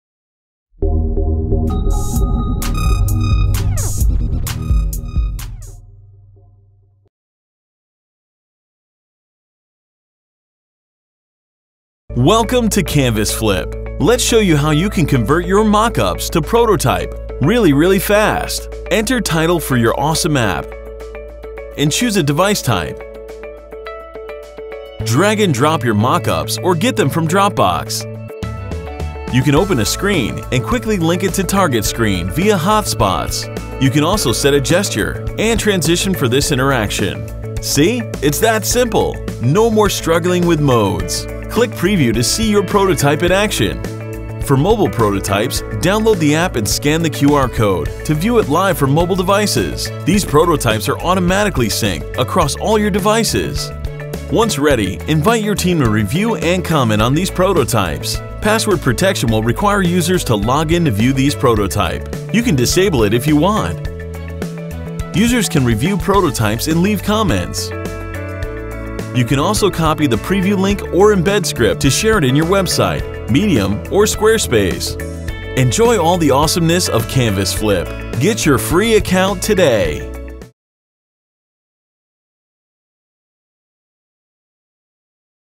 CanvasFlip: Phân tích UX ngay ở trên Prototype của bạn.
Với các bạn UX & UI Designer chắc sẽ không lạ lẫm gì các tool chuyên sử dụng để tạo ‪#‎Prototype‬, ví dụ như các tool nổi tiếng như ‪#‎Invisionapp‬, ‪#‎Marvel‬, ‪#‎Moqups‬ hay ‪#‎Axure‬. Nhưng hôm nay UX Hacker lại muốn giới thiệu tới các bạn thêm 1 tool làm Prototype khác mà mình cảm thấy rất tâm đắc khi sử dụng đó chính là CanvasFlip. Vậy điểm khác biêt của nó là gì mà UX Hacker lại ưu ái quá vậy? Mình sẽ chia sẻ cho các bạn một số lí do như dưới đây:

Ngoài những tính năng như tạo Prototype, quản lý Assets hay To-Do List như các tool khác đều có thì điều đầu tiên mà UX Hacker muốn nói đến chính là tính năng dành cho việc phân tích UX ngay ở trên Prototype của bạn bằng những công cụ như sau:

☀ Get Actionable Feedback On UX From “Real Users”: CanvasFlips giúp bạn đưa Prototype đến Real User (ở đây có thể là team của bạn, Khách hàng hoặc chính là ➡ Actual Target Audience ⬅ mà bạn đang muốn hướng tới.)
☀ Session Replay: Ngay lập tức, CanvasFlip sẽ ghi lại đoạn video người dùng đang sử dụng prototyping từ xa và cho bạn xem.
☀ Conversion Funnel: Còn hơn cả video, CanvasFlip sẽ hỗ trợ bạn xác định ngay những screens nào có nguy cơ bị Drop-off cao hơn khi User đang sử dụng trong UX Flow của App.
☀ Interaction Heatmap: Với Heatmap thì bạn sẽ thấy được nơi mà User tương tác nhiều nhất trên Prototype của mình.

Với các tính năng tuyệt vời như trên thì các bạn có thể thoải mái tìm và sửa chữa ngay những vấn đề về Usability trong Prototype, cũng như là thiết kế của bạn.

‪#‎GrowthTogether‬ ‪#‎UXDesign‬ ‪#‎UXAnalytics‬ ‪#‎Prototyping‬

☀ Vậy thì còn chờ gì nữa mà không trải nghiệm CanvasFlip ngay hôm nay